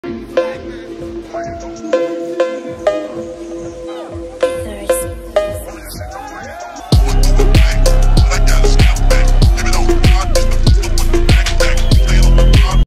Daniel laizan beast mode combo
The time Daniel laizan came to canda in 2022